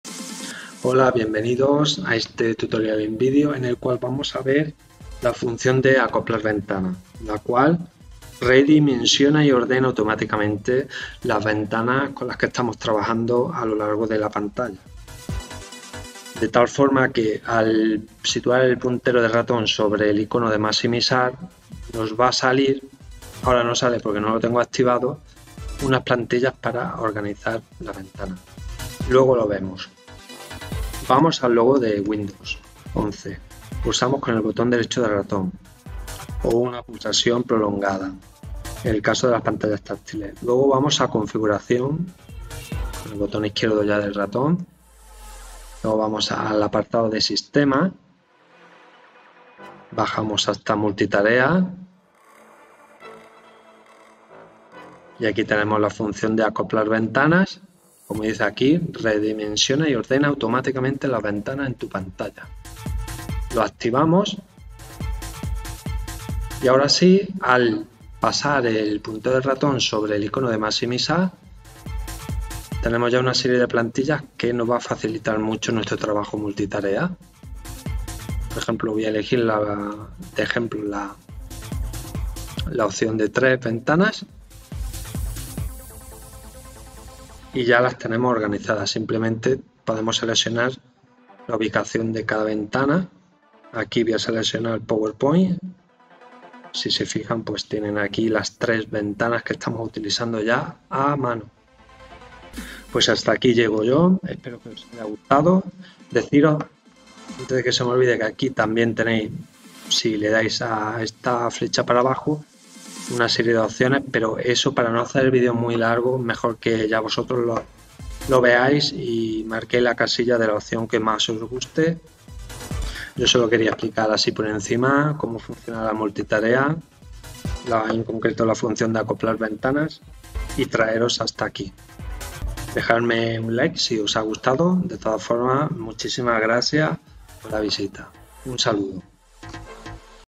Como activar y desactivar la función ACOPLAR VENTANAS 👉 Windows 11
Tutorial en vídeo en el que enseño como se desactiva y activa la opción de acoplar ventanas en Windows 11, que redimensiona y ordena automáticamente las ventanas en tu pantalla.

Mostrar las opciones de anclaje junto a las ventanas ancladas
Mostrar diseños de acoplamiento al mantener el mouse sobre el
botón Maximizar de una ventana
Mostrar diseños de acoplamiento de los que forma parte la
aplicación al mantener el mouse sobre los botones de la barra de
tareas
Al arrastrar una ventana, dejarme acoplarla sin arrastrar hasta el
borde de la pantalla
Cambiar automáticamente el tamaño al acoplar una ventana para
rellenar el espacio disponible
Cambiar el tamaño de las ventanas acopladas adyacentes al
cambiar el tamaño de una ventana acoplada